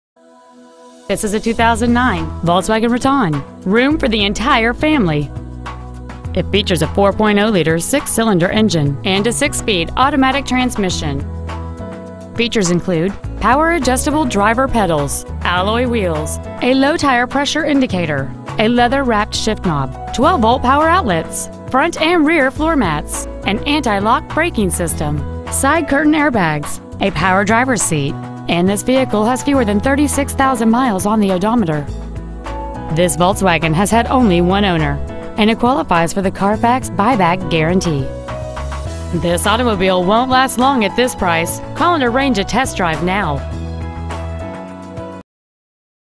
Preowned 2009 Volkswagen Routan Miami FL
We are proud to present this 2009 Volkswagen Routan .

 Year : 2009
 Make : Volkswagen
 Model : Routan
 Engine : gas v6 4.0l/
 Trans . : automatic
 Exterior : blue
 Miles : 35,710
 Interior : tan

 Lehman Mazda